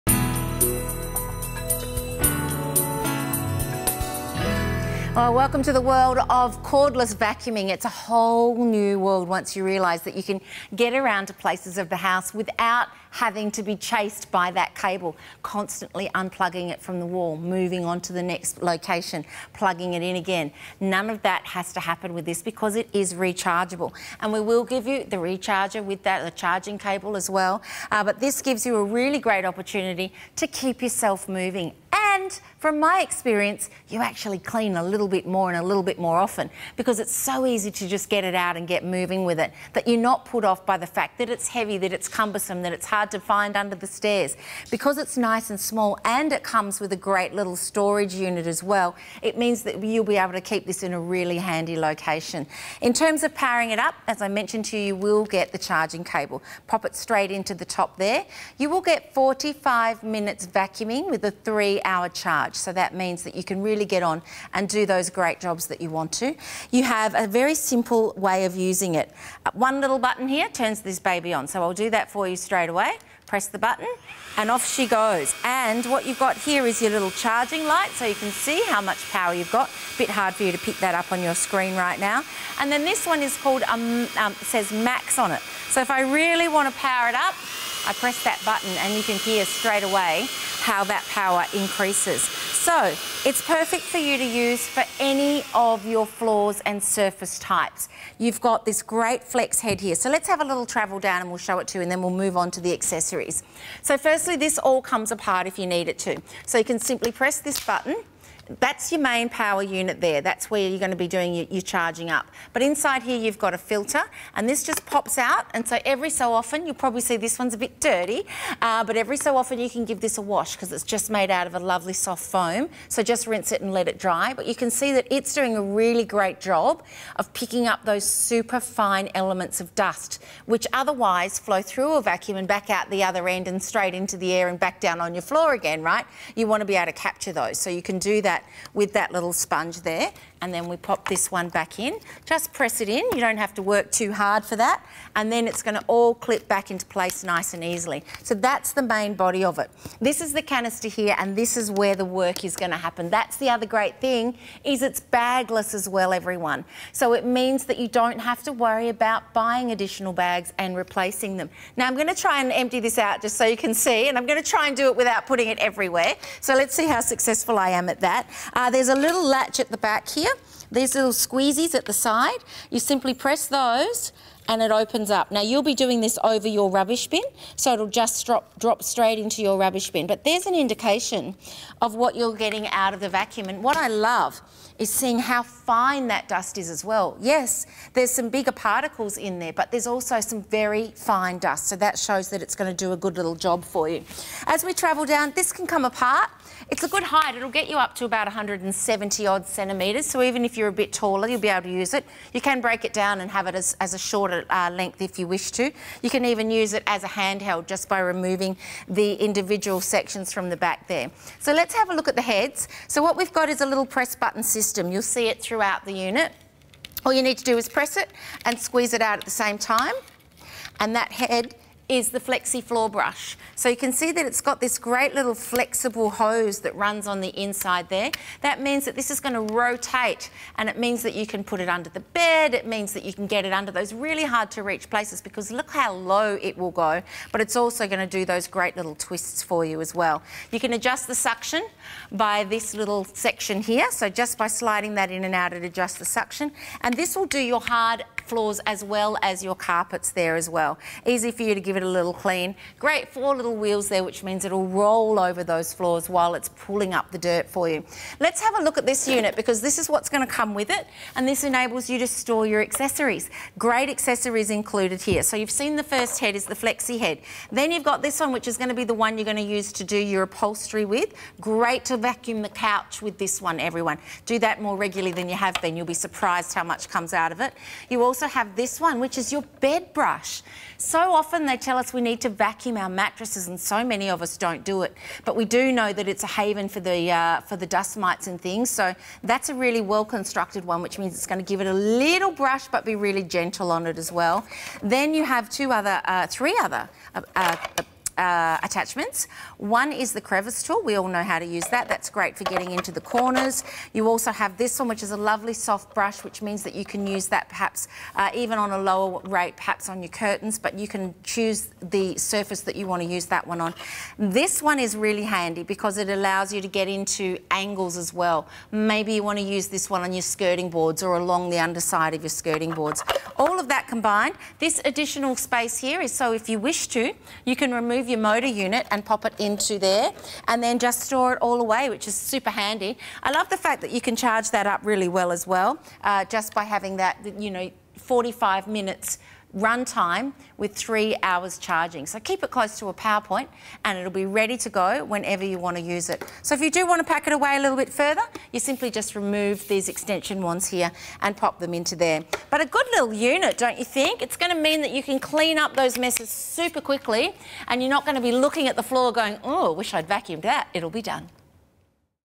Innovations Rechargeable Cyclone Vacuum Cleaner
Winner of a prestigious Singapore Design Award, this light yet powerful vacuum cleaner extends to 170 cm long so you can reach into every corner from floor to ceiling. There are accessories for surfaces from hard floors and carpets to mattresses as well as one for awkward crevices and you can switch between two suction strengths. Powered for 45 minutes from a 3-hour charge it’s easy to empty, store and clean.